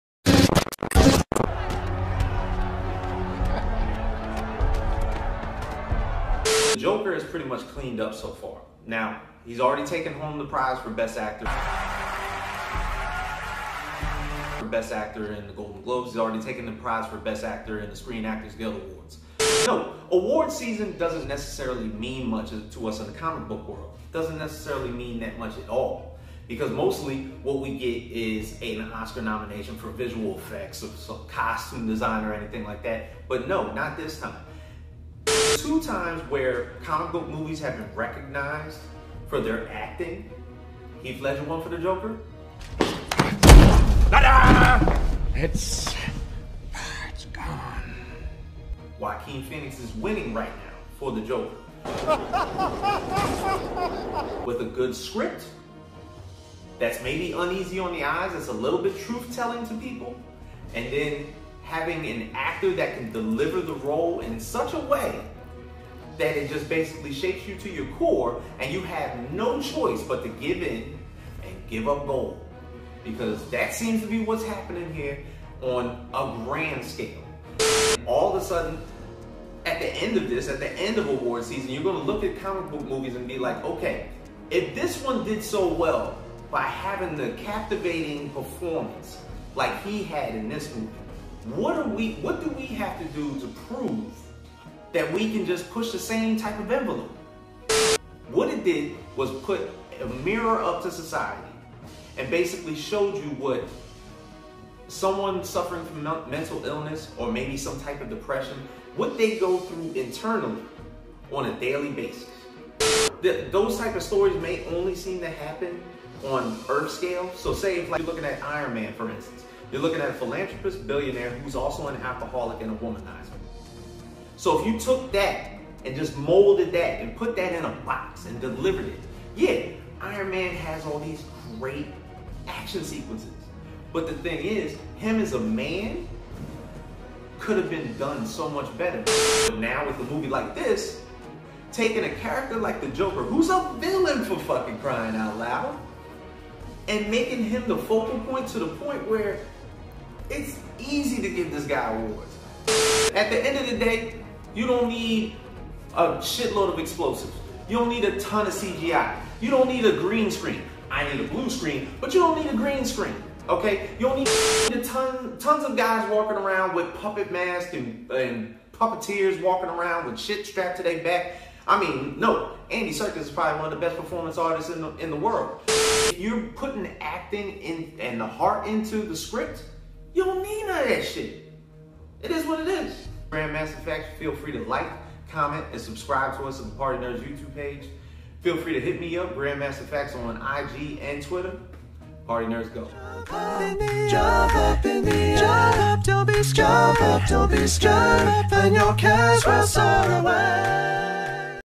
JOKER WINS
James Grandmaster FAQs talks about Joaquin Phoenix winning the Oscar for Best Actor and why this is IMPORTANT to Comic Book Films in Hollywood moving forward!

Acapella segment by @Sam Robson

*All Video clips are owned by their respective companies & production studios.